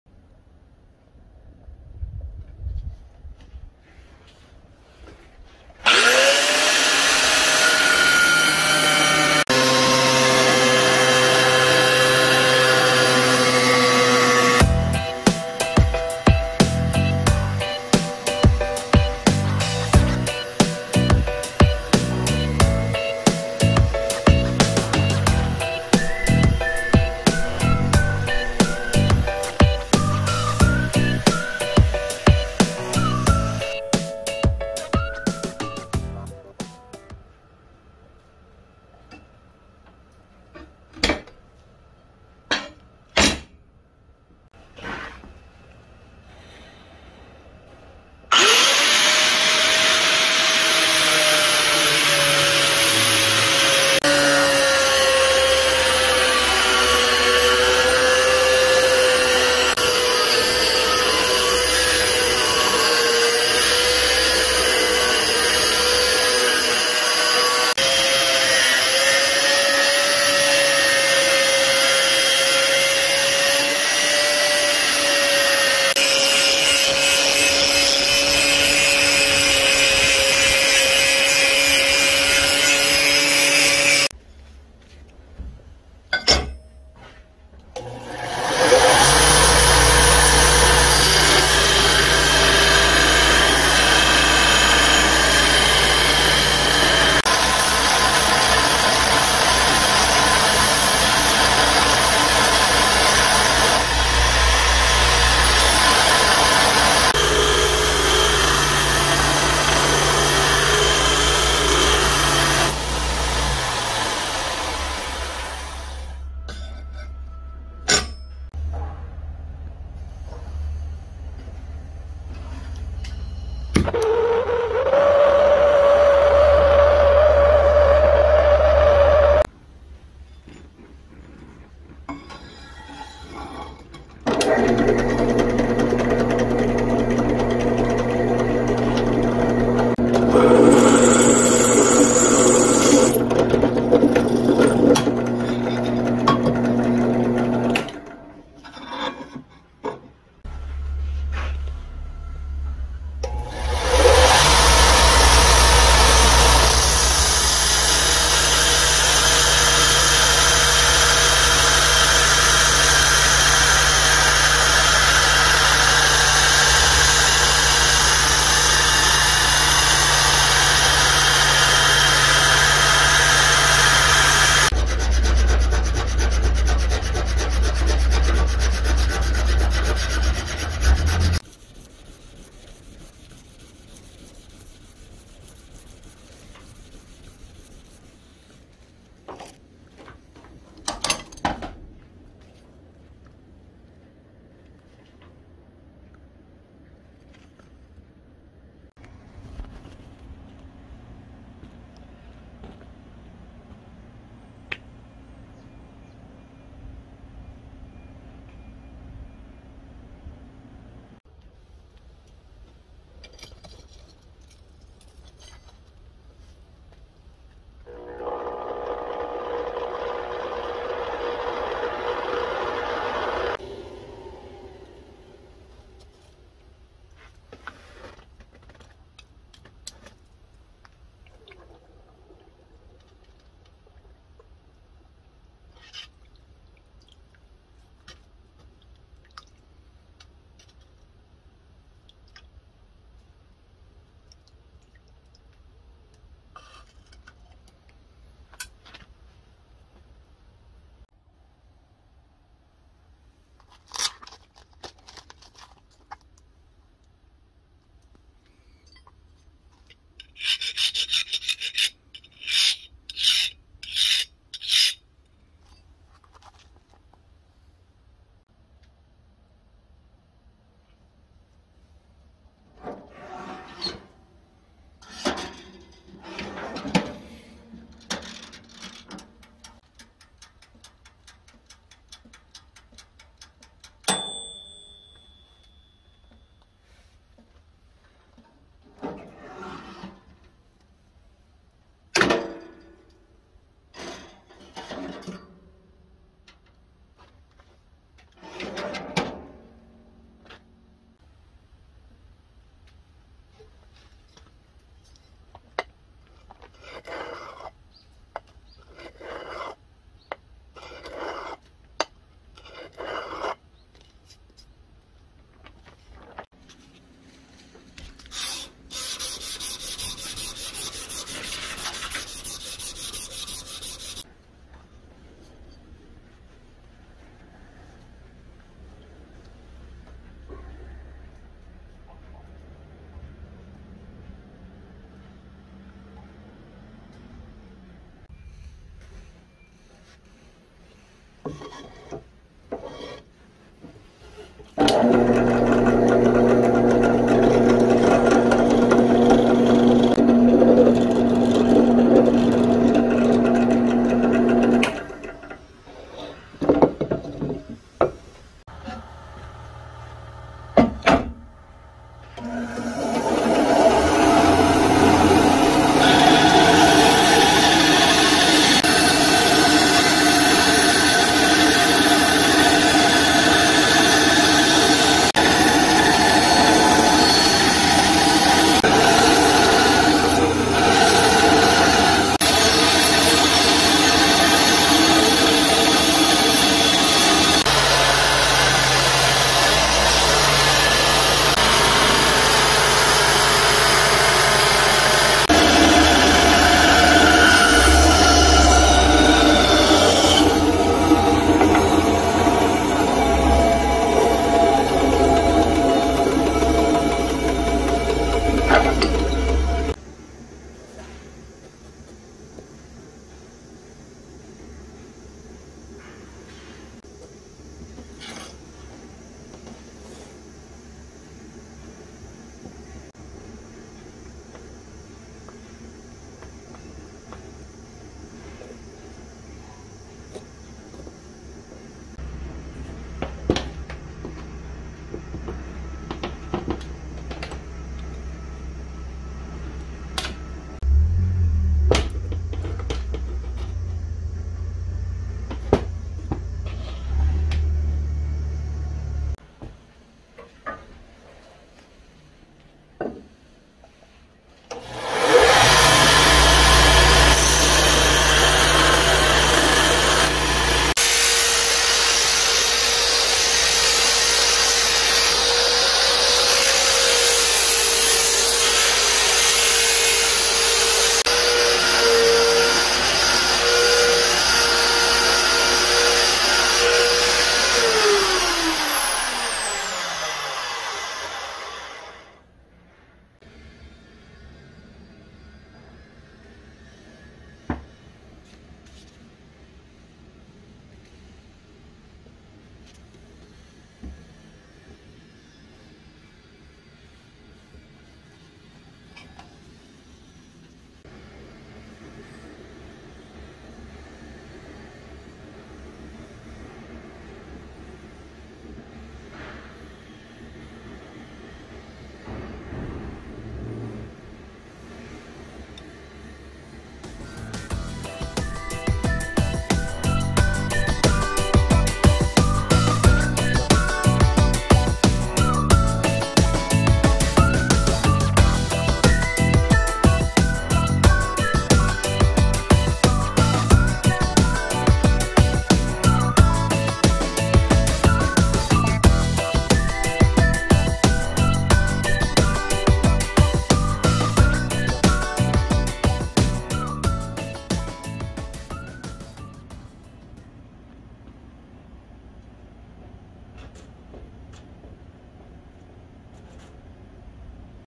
Making a camping knife
Good day!
This knife is a camping knife based on the Tanto knife.
This knife is a knife made by cutting a huge saw blade.
The length of the blade is about 13cm and the thickness is 5mm.
I think it would be good to use it for camping.